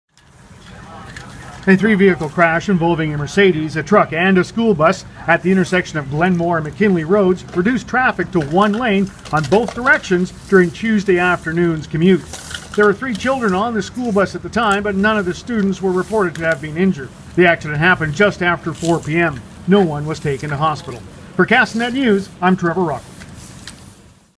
School bus crash
A school bus and two other vehicles collided Tuesday Feb. 25 in Kelowna